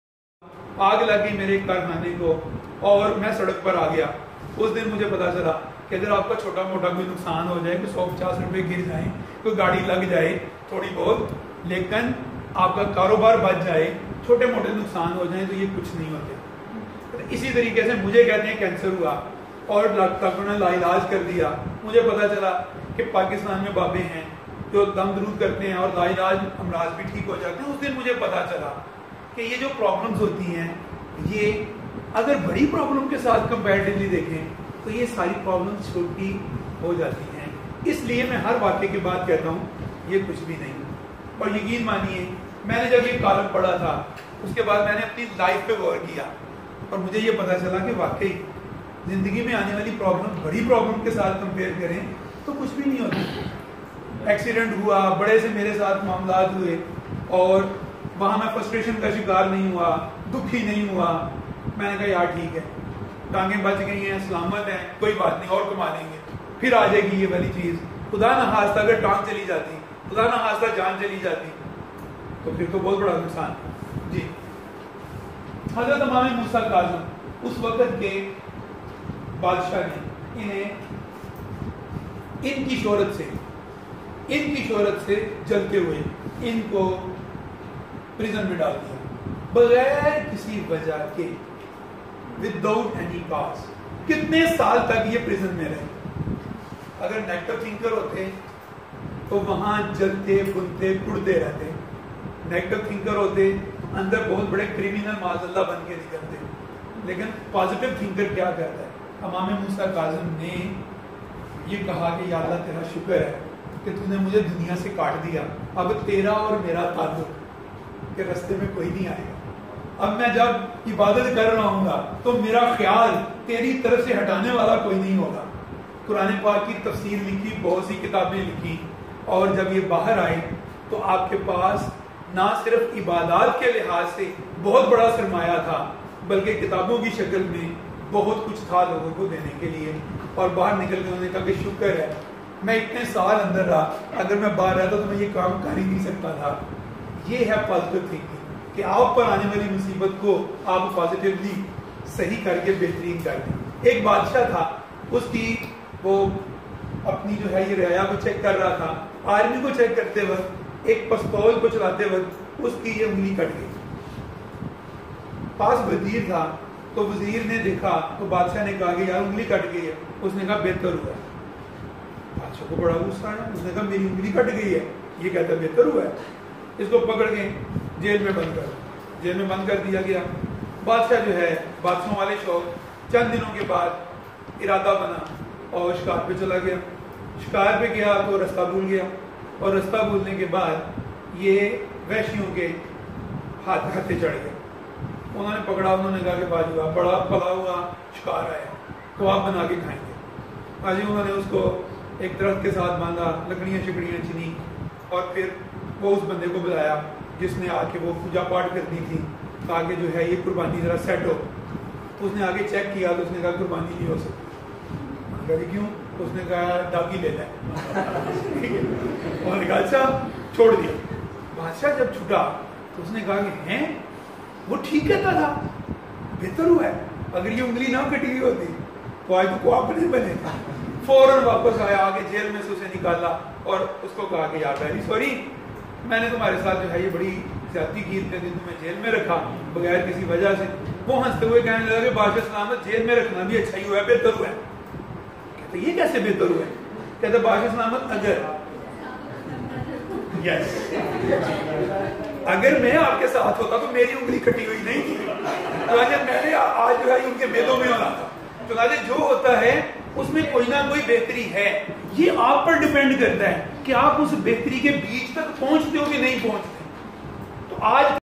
Positive mental attitude by Zulfiqar Mughal part 17
Punjab Group of Colleges Narowal organized a training session for their teachers during ramadan. They requested Zulfiqar Mughal to come and give a brief lecture
for the teachers.
MD of the College Mr. Zahid Sulehri and the principal was also present in the training with their copies and pen.